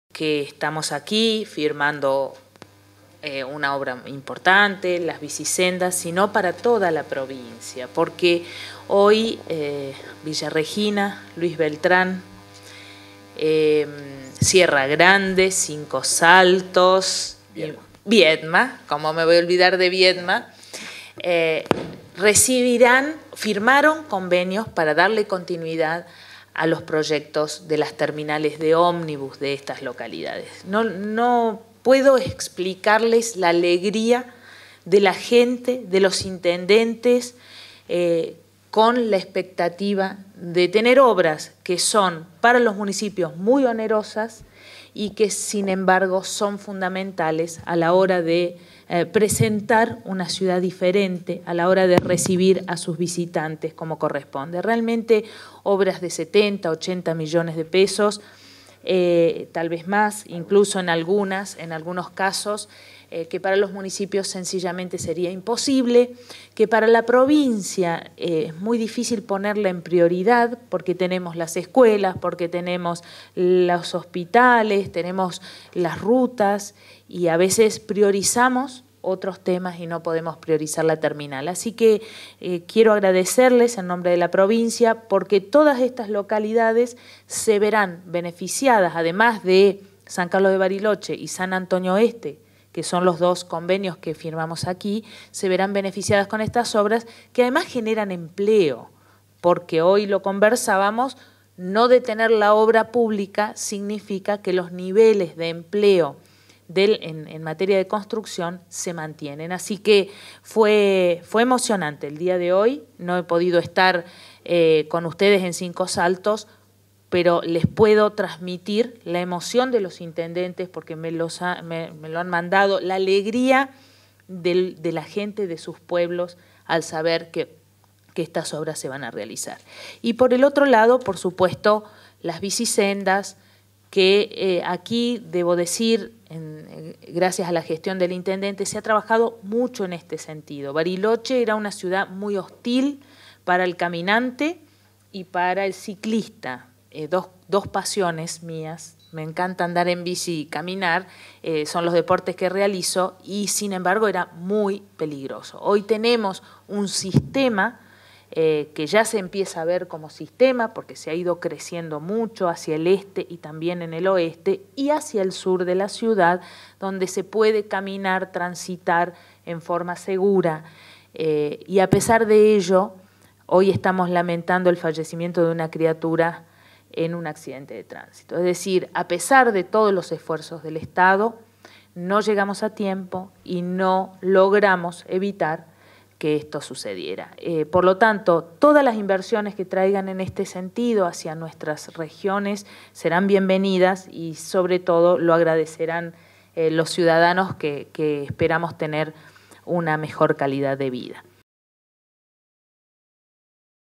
Carreras y Meoni firmaron convenio para la construcción de la nueva terminal de ómnibus de Bariloche
Arabela Carreras, Gobernadora de Río Negro